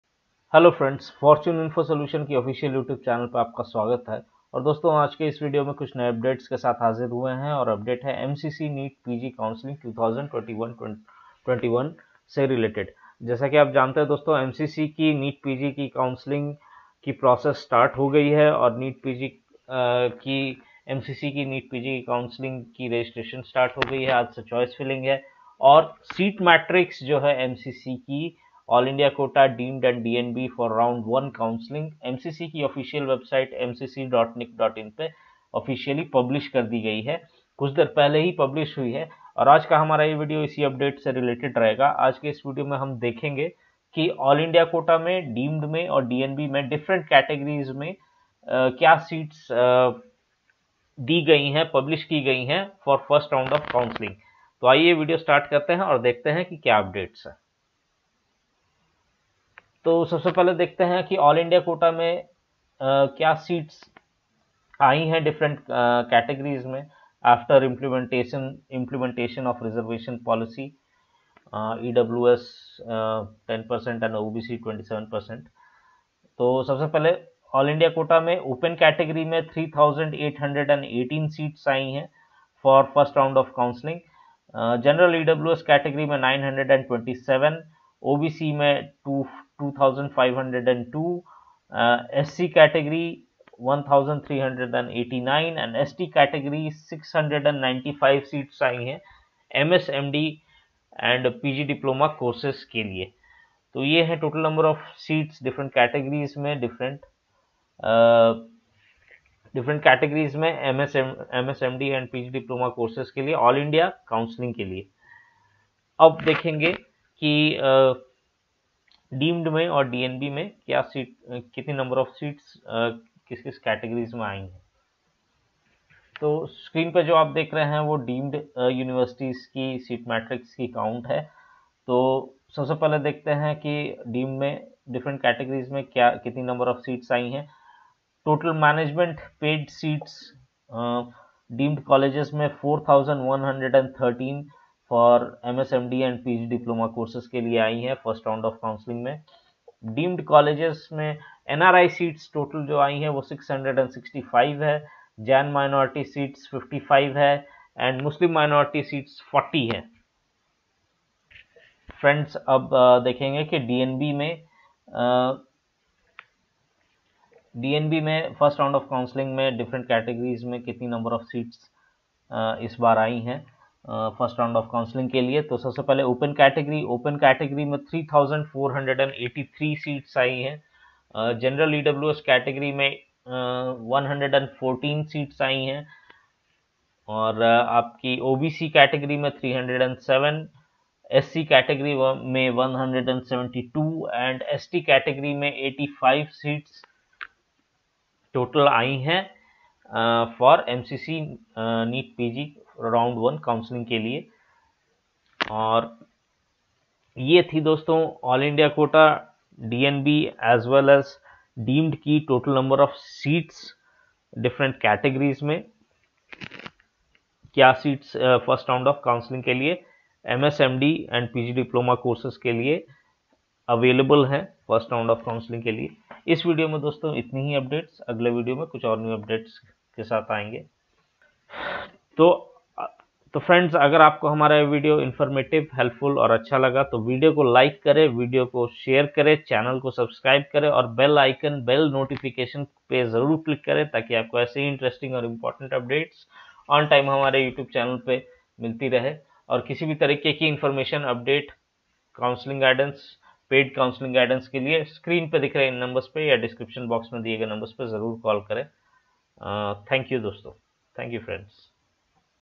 🔥MCC NEET PG ROUND1 COUNSELLING SEAT MATRIX FOR AIQ/DNB/DEEMED COLEGES FOR MS/MD/DIPOMA PUBLISHED🔥
This video contains information regarding Mcc Neet pg counselling seat matrix for round1 counselling 2021-22 session.
For any further information update or counselling guidance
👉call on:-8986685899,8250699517